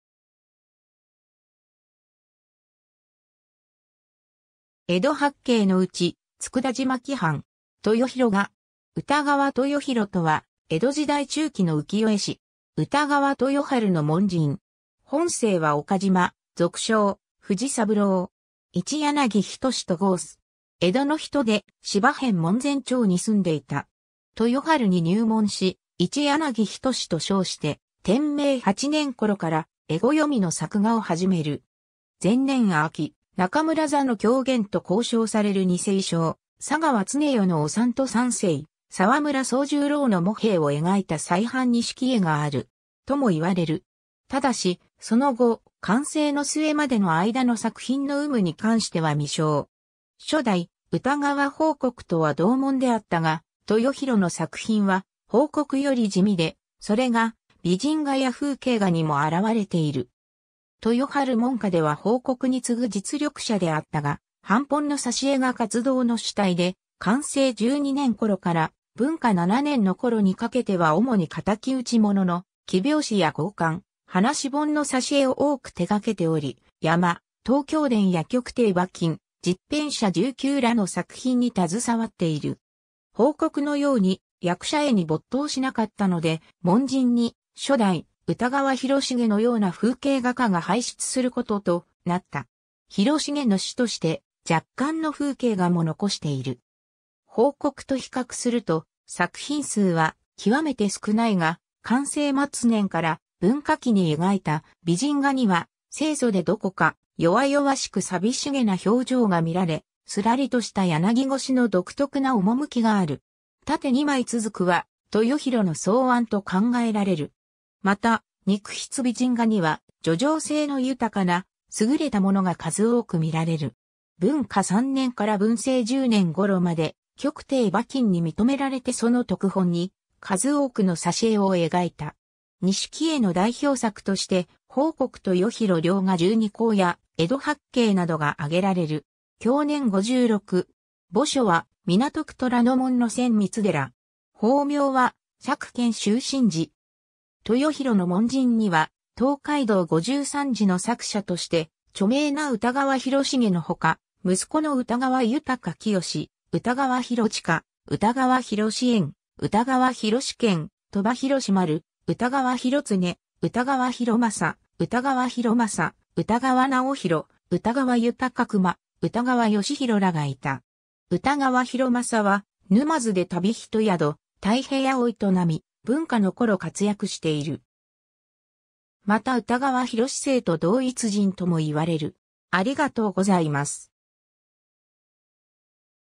歌川豊広
「江戸八景」のうち「佃島帰帆」。
豊広画。
 歌川 豊広（うたがわ とよひろ、安永3年〈1774年〉 - 文政12年12月21日〈1830年1月15日〉）とは、江戸時代中期の浮世絵師。
 歌川豊春の門人。
本姓は岡島、俗称藤三郎。
一柳斎と号す。
江戸の人で芝片門前町に住んでいた。
 豊春に入門し一柳斎と称して天明8年（1788年）頃から絵暦の作画を始める。
前年秋、中村座の狂言と考証される二世小佐川常世のおさんと三世沢村宗十郎の茂兵衛を描いた細判錦絵がある。
ともいわれる。
ただし、その後、寛政の末までの間の作品の有無に関しては未詳。
初代歌川豊国とは同門であったが、豊広の作品は豊国より地味で、それが美人画や風景画にも現れている。
豊春門下では豊国に次ぐ実力者であったが、版本の挿絵が活動の主体で、寛政12年（1800年）頃から、文化7年（1810年）の頃にかけては主に敵討物の黄表紙や合巻、噺本の挿絵を多く手がけており、山東京伝や曲亭馬琴、十返舎一九らの作品に携わっている。
豊国のように役者絵に没頭しなかったので、門人に初代歌川広重の様な風景画家が輩出することとなった。
広重の師として若干の風景画も残している。
豊国と比較すると作品数は極めて少ないが、寛政（1789年 - 1801年）末年から文化（1804年 - 1818年）期に描いた美人画には、清楚でどこか弱々しく淋しげな表情が見られ、すらりとした柳腰の独特な趣がある。
縦二枚続は豊広の創案と考えられる。
また、肉筆美人画には叙情性の豊かな優れたものが数多く見られる。
文化3年（1806年）から文政10年（1827年）頃まで、曲亭馬琴に認められてその読本に数多くの挿絵を描いた。
錦絵の代表作として、「豊国豊広両画十二候」や「江戸八景」などがあげられる。
享年56。
墓所は港区虎ノ門の専光寺。
法名は釈顕秀信士。
 豊広の門人には「東海道五十三次」の作者として著名な歌川広重のほか、息子の歌川豊清、歌川広近、歌川広演、歌川広兼、鳥羽広丸、歌川広恒、歌川広昌、歌川広政、歌川直広、歌川豊熊、歌川芳広らがいた。
歌川広昌は沼津で旅人宿「太平屋」を営み、文化のころ活躍している。
また歌川広政と同一人ともいわれる。